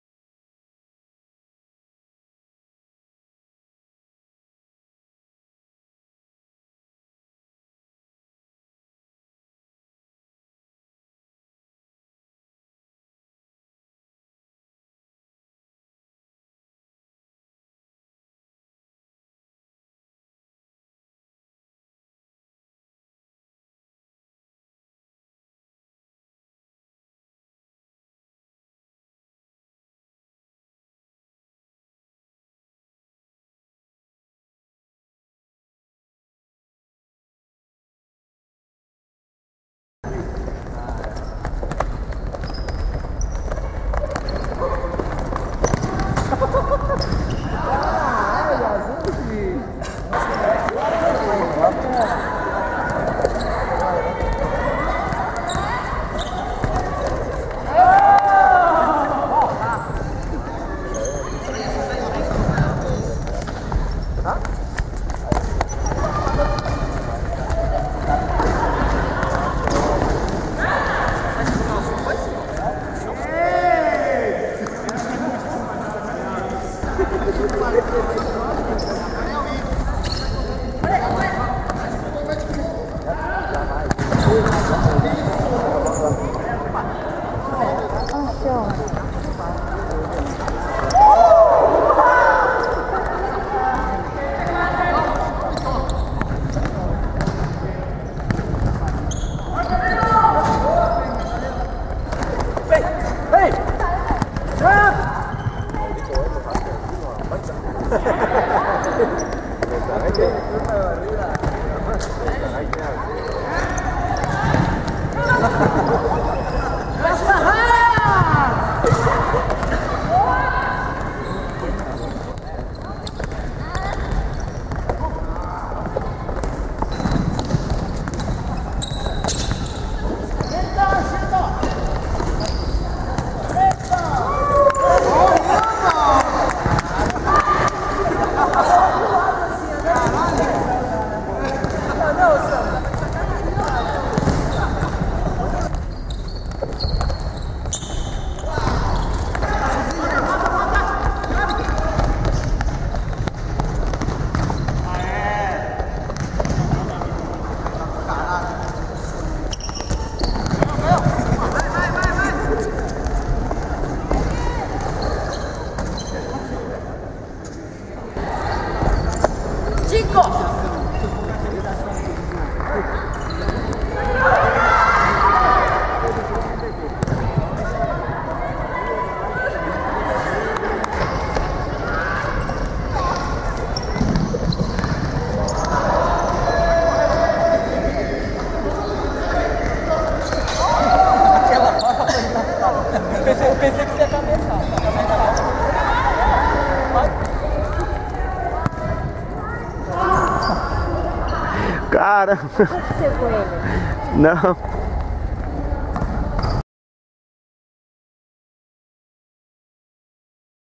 Turma do Futsal Golden Week (final) Japan 2013
Finais dos lances das 4 horas... Zé, Carlos, Ossamu,Tico, Marcão, Alegria, Nagano, Daniel, Yasushi, Hugo, Daniel, Perninha, David, Chaves, Júnior, Sasaki, Muramatsu, Kasahara, Simão
Hiratsuka Sun Life Arena, Kanagawa, Japan, Mai.2013